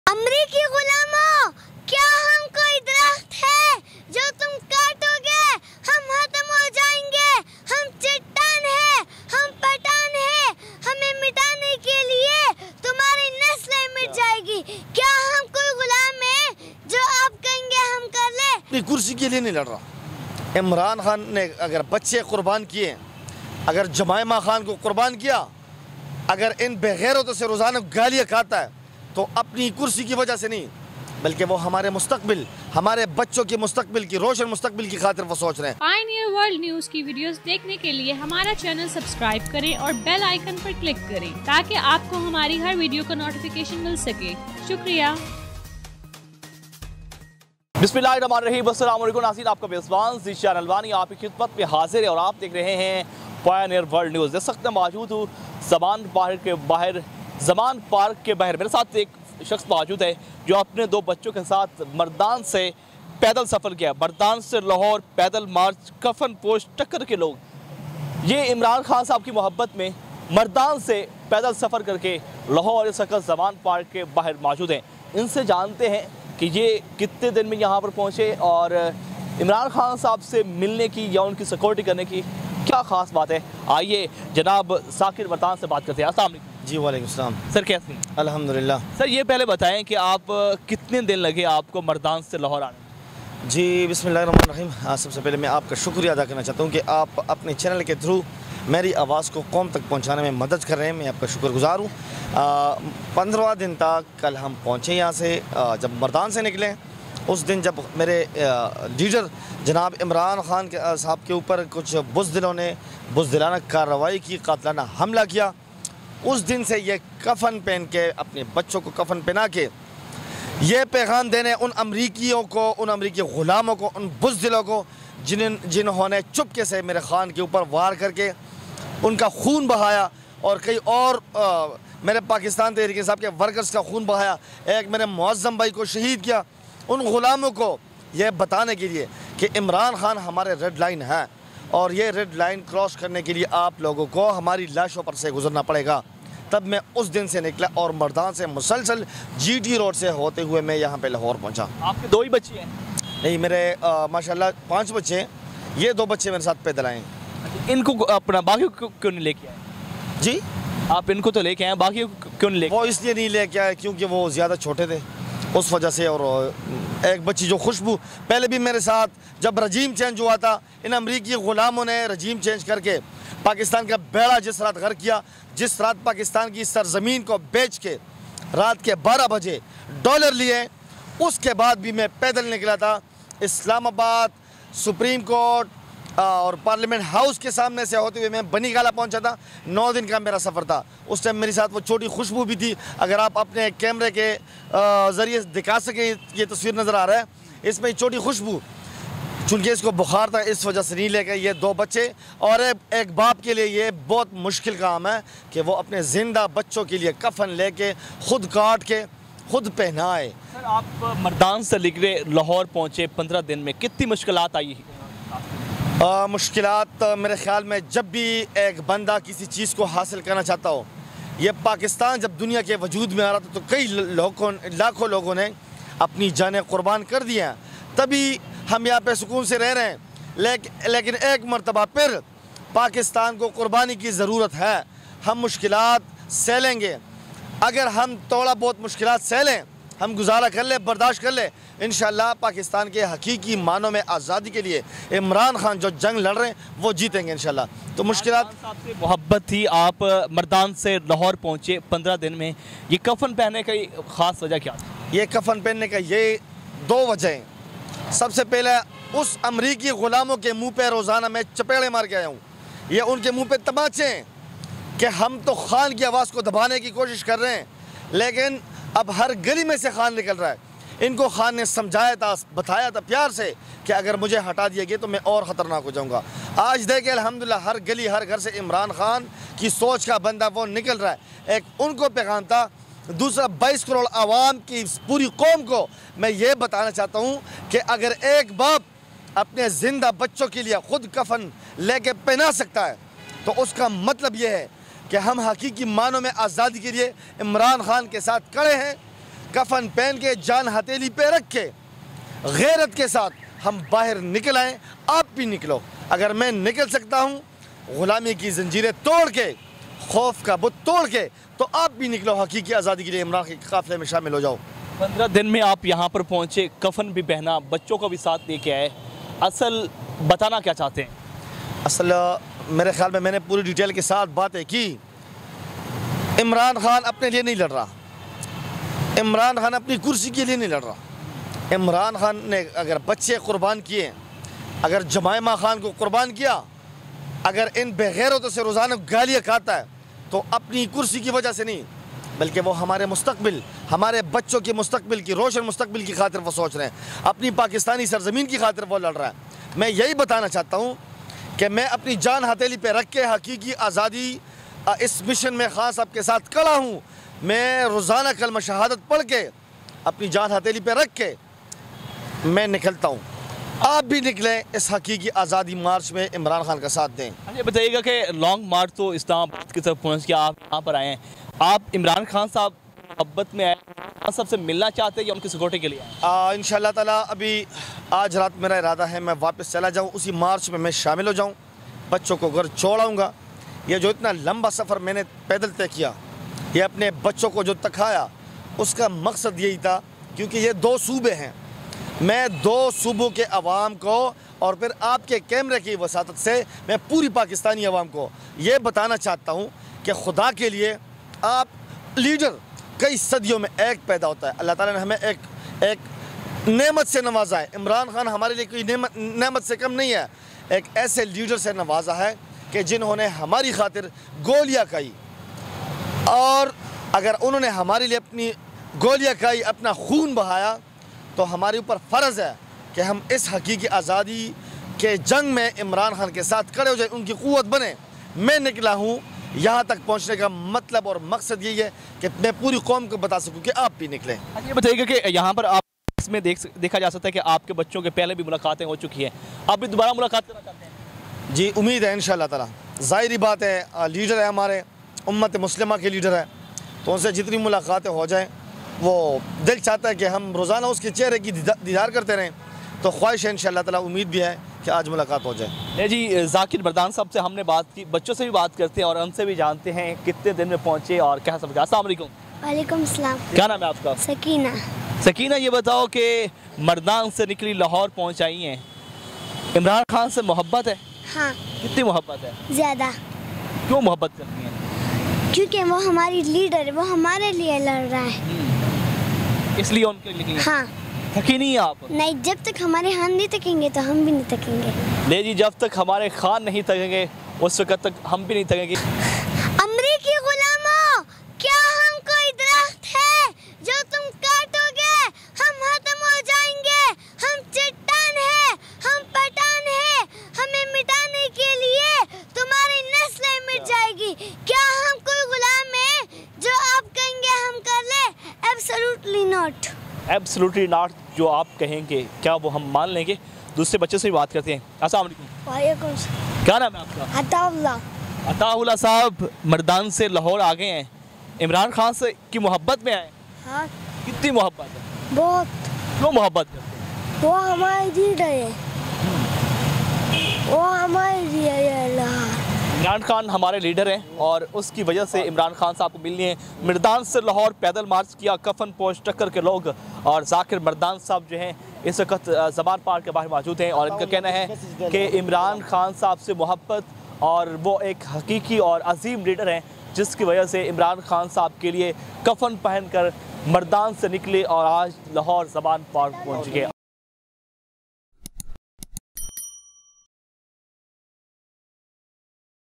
Imran Khan Ki Muhabat Mien Mardan se Do Bacchon K Sath Padal Kafan Pehan Kar Zaman Park Phunch Gaya
Host : Zeeshan Almani